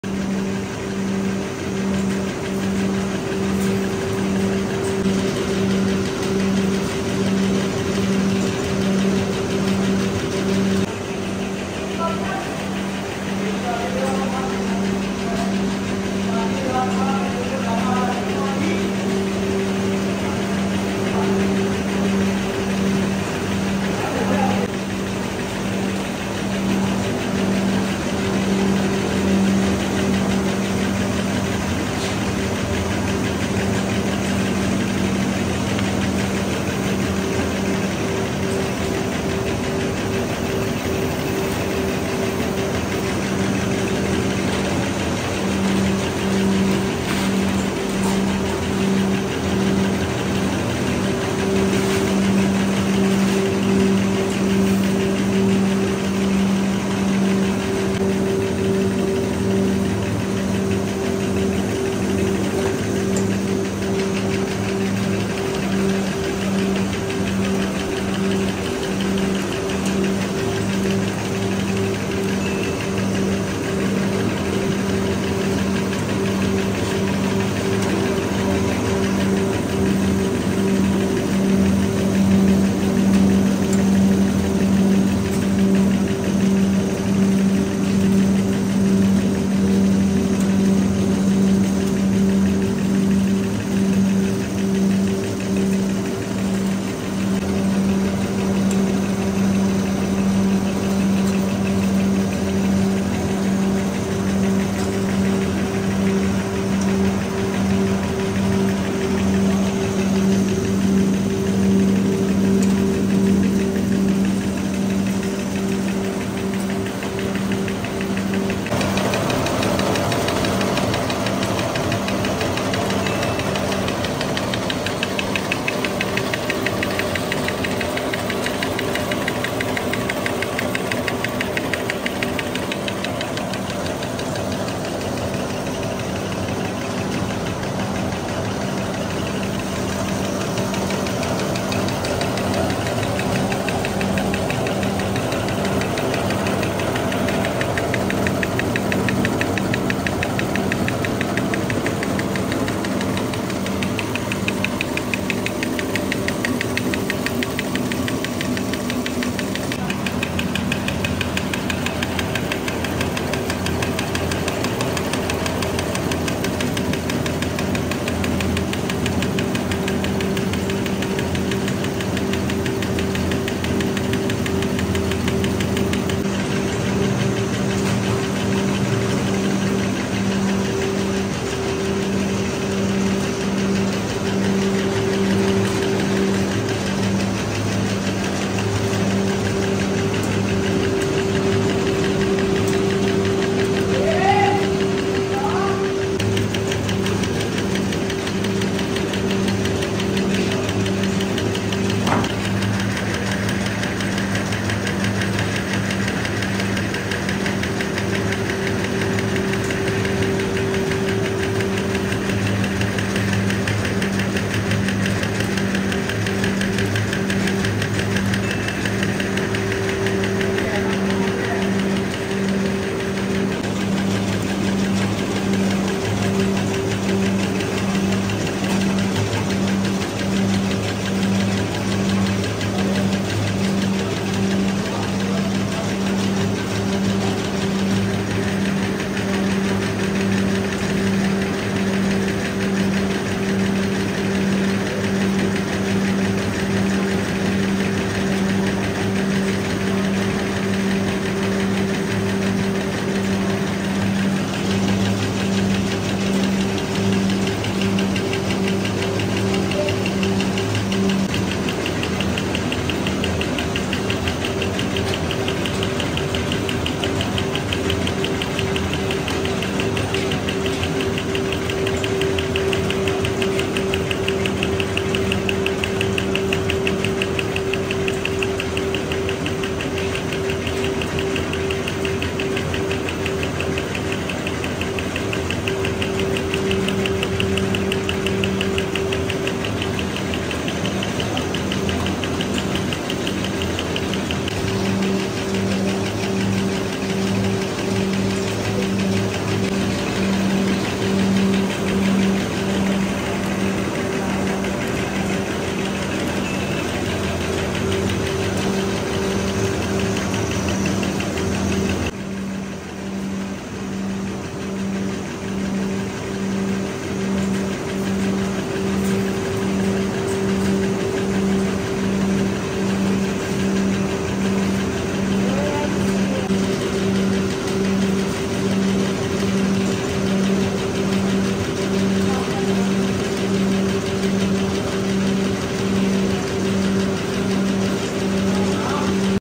Process Of Making Straws in Asia | Straws Mass Production Factory
Have you ever wondered how straws are made? In this video, we take you inside a straws mass production factory in Asia, where millions of straws are manufactured every day. You will see the whole process of making straws, from raw materials to finished products, and learn about the different types, sizes, colors, and shapes of straws. You will also discover some interesting facts and trivia about straws, such as their history, environmental impact, and alternative uses. Watch this video and be amazed by the fascinating world of straws!